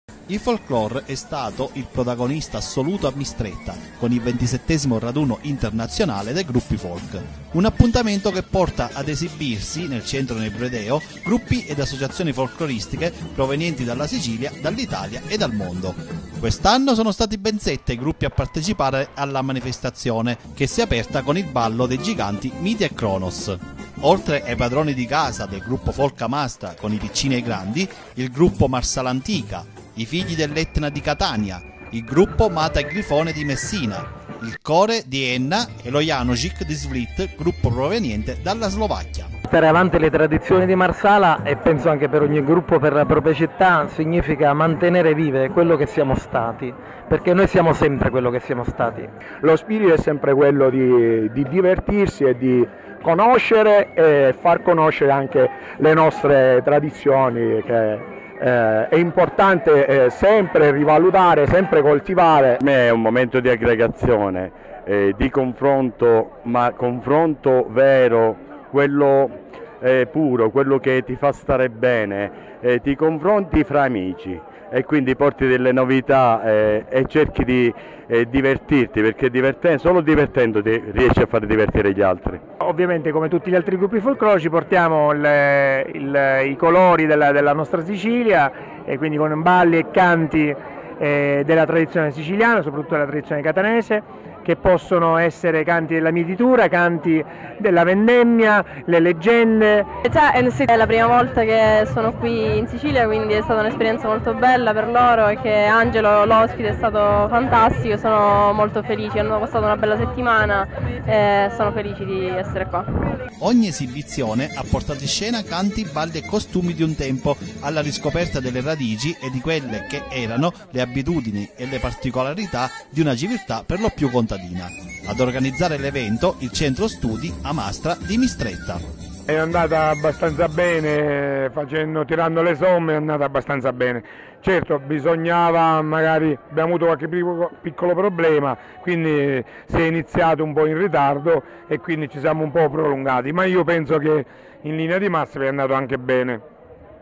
XXVII raduno internazionale del folklore a Mistretta
Si è ripetuto uno degli appuntamenti fissi dell'estate amastratina che ha visto quest'anno la partecipazione di ben 7 gruppi folkloristici